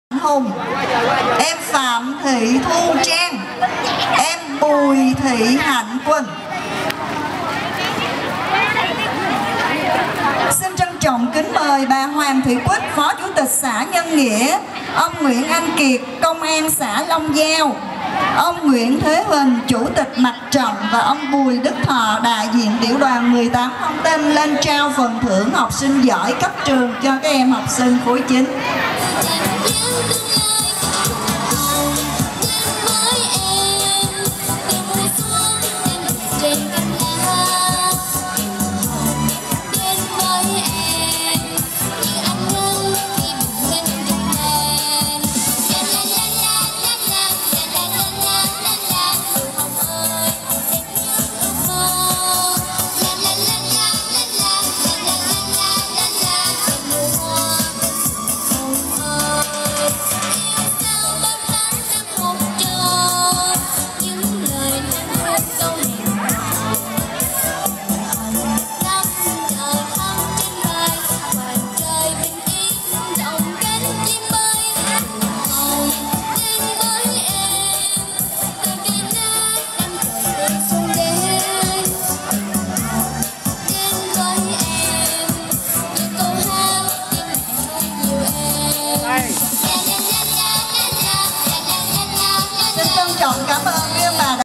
Phát thưởng HSG Khối 9 - Chu Văn An 2012
Lễ Tổng Kết - Phát thưởng HSG Khối 9 , Trường THCS Chu Văn An, Nhân Nghĩa, năm học : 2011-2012